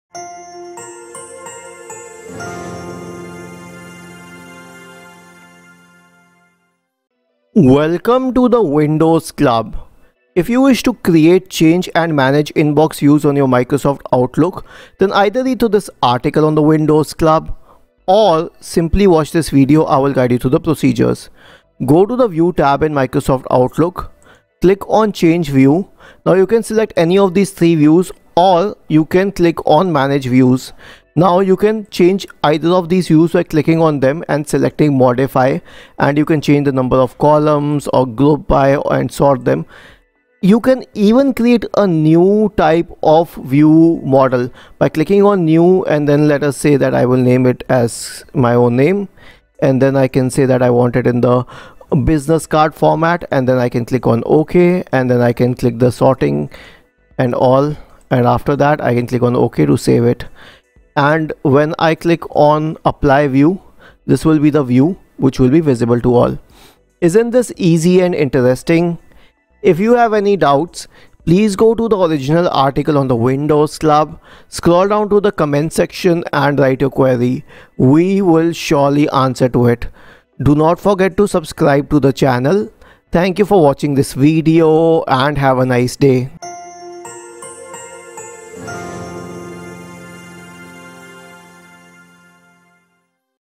How to Create, Change and Manage Inbox Views in Microsoft Outlook
Views in Microsoft Outlook give you different layouts in terms of the way the items in the folders look. In this guide, we see how you can create and change the Views of the Inbox folder in Microsoft Outlook.

Timecodes:
0:00 Intro
0:21 Procedure through Outlook